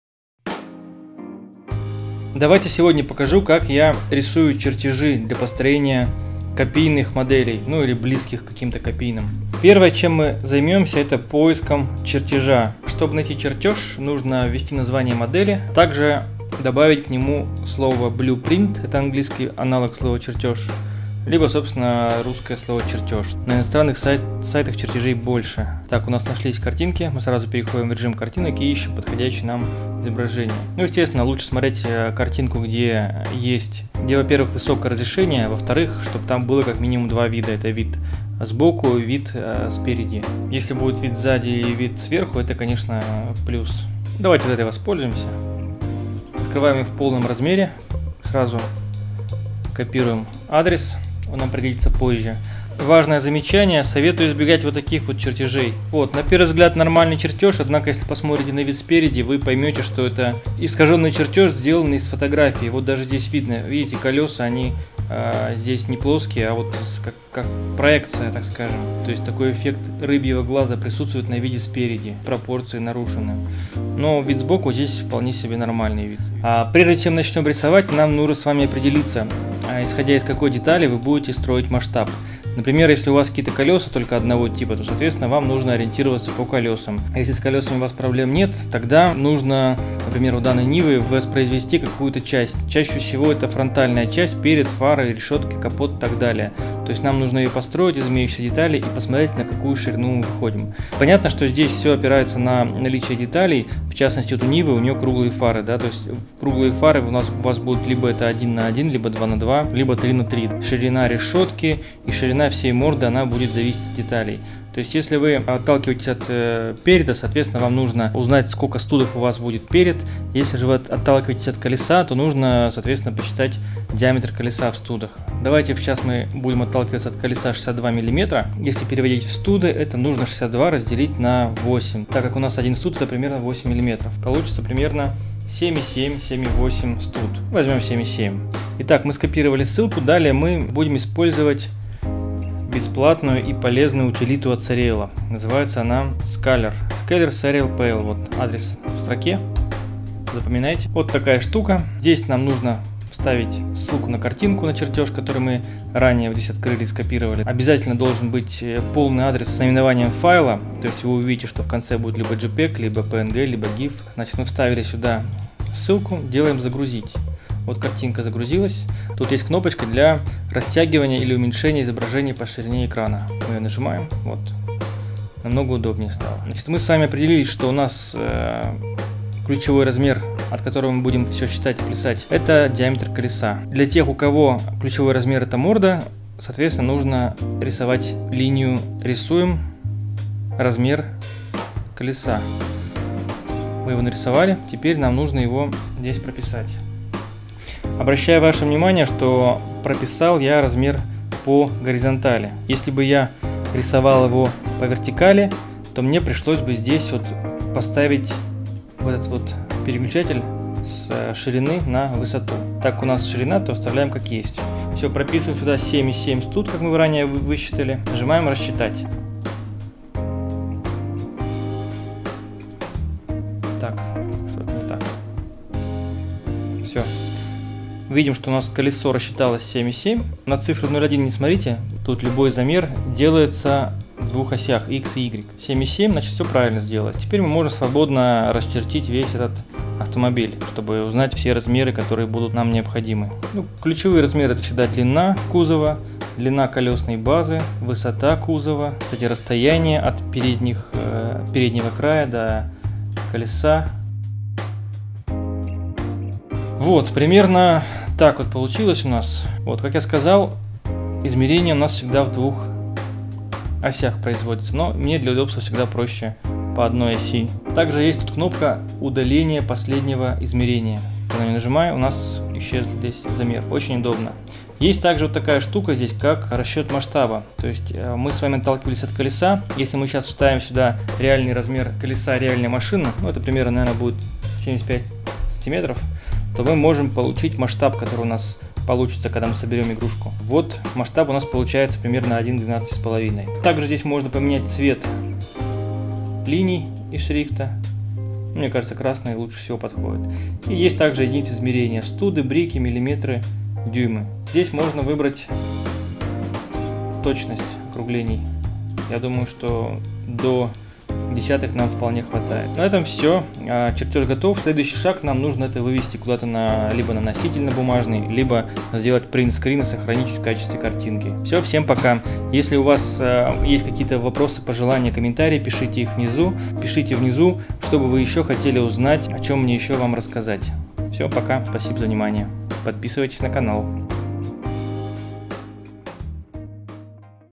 [VLOG #3] Расcчитываем размеры модели. Чертеж. Using Scaler Tool by Sariel. Scaler tool.
При планировании модели, особенно если речь идет о реальном прототипе, важно заранее рассчитать все размеры и пропорции. Даже если вы все в голове заранее прикинули - скорее всего вы ошибетесь) Используем классный инструмент от  Sariel - scaler.sariel.pl

While planning model it important to count all sizes to get right proportions in the end. Building ïn your head" usuaully bad and give you failed projects. So dont be lazy to count! Sariel's tool is very useful and simple!

Спасибо Sariel за полезную штуку!
Thanks to Sariel for useful tool!